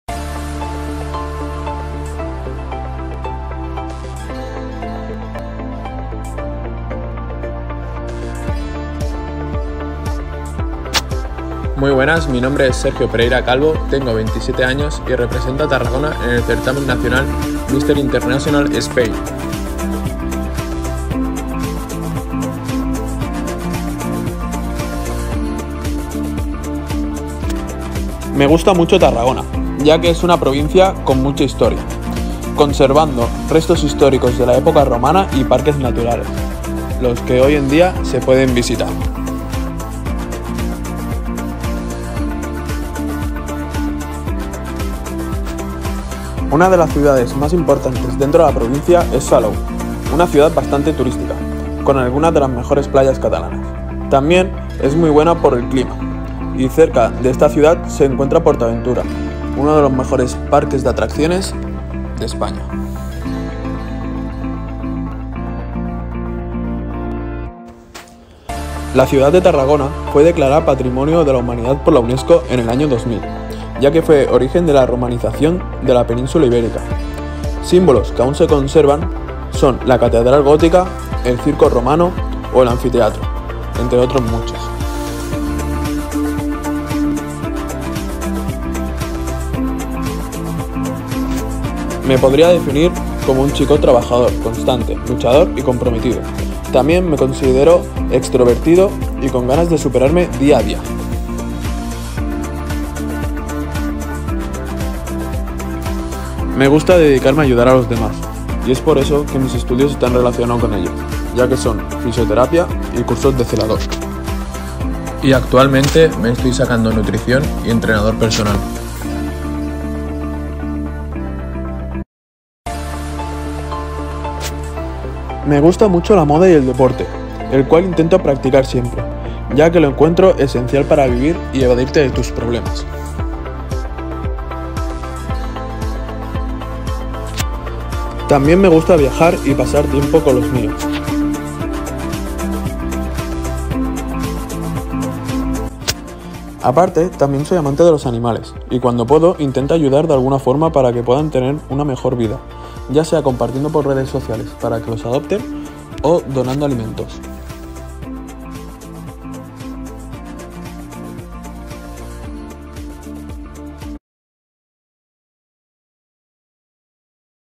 MRI. ESPAÑA 2022 | Descubriendo a... TARRAGONA
TARRAGONA
Sergio Pereira representara a la Provincia de Tarragona en el certamen Mister Internacional España 2022.

GALA MISTER INTERNACIONAL ESPAÑA 2022
Dia 17 de Junio a las 21:00h. Can Xaubert  (Pineda de Mar)

 ¡ Y no te pierdas nada! 👁️